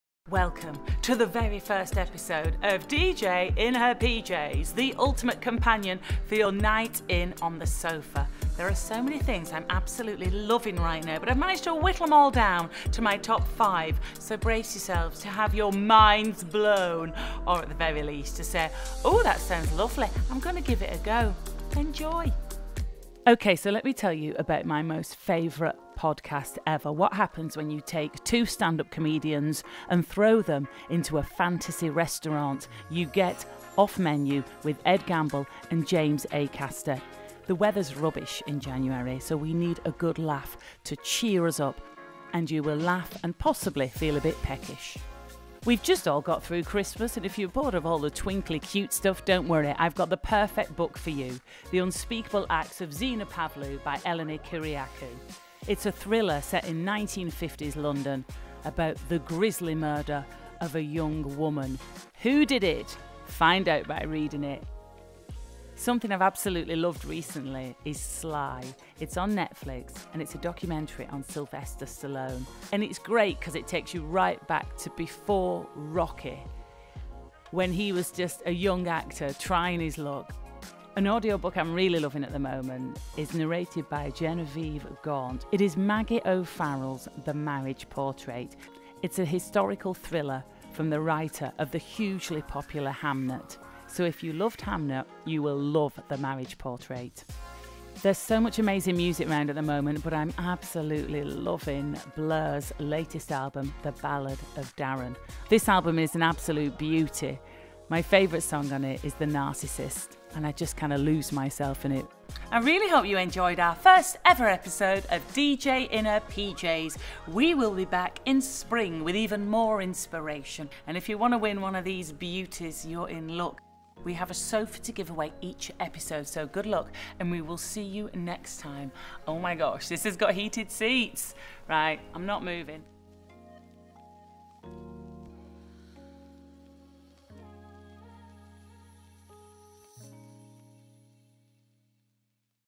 Sofology x Sara Cox
Introducing episode 1 of our seasonal 'DJ In Her PJs' social campaign with the well-read, well-watched Sara Cox! Thank you to Sofology and YMU for bringing us into the fold.